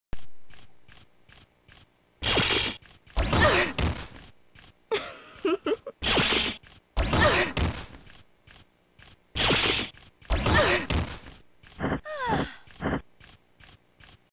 Electrocuted Valerie Vultures (Navetsea)
Original Character: Valerie Vultures
Author: Navetsea

I actually forgot if Navetsea himself did the shock, or if someone did it for him originally...I think it was Tsunamidusher that did it? Hmm.